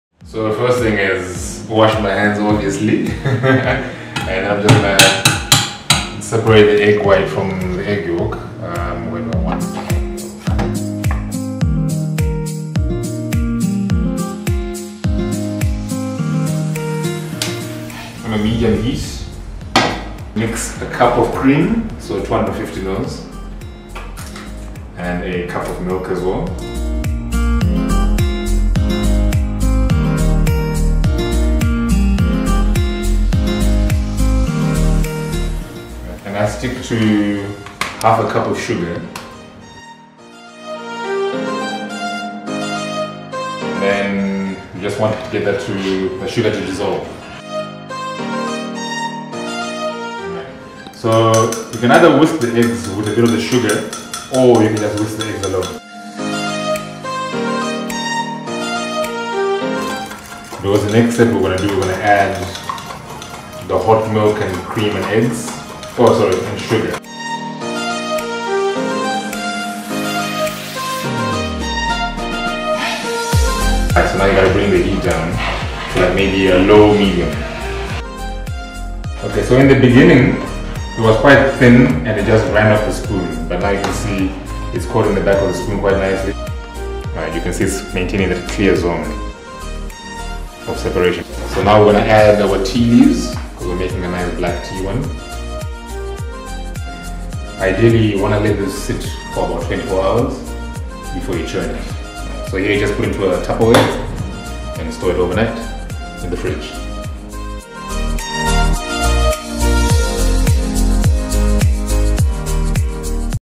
Tapi Tapi frozen desserts: how it's made
For Dr Tapiwa Guzha, enjoying frozen desserts should always go along with a story. “Food is what brings people in, but then you want them to share a story with you - that’s what I’m interested in,” says Dr Guzha, currently a research fellow in the department of genetics at Stellenbosch University. Read more on MatieMedia.org.

Video by Ané van Zyl